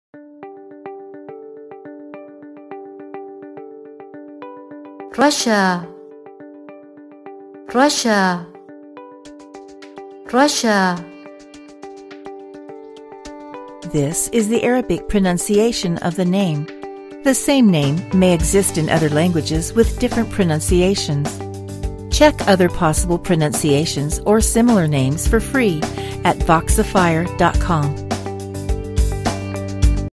How to Pronunce Rasha (راشا) in Arabic - Voxifier.com
This is the Arabic pronunciation, gender, and phonetics recording of the name Rasha (راشا). The same name may exist in other languages with different pronunciations.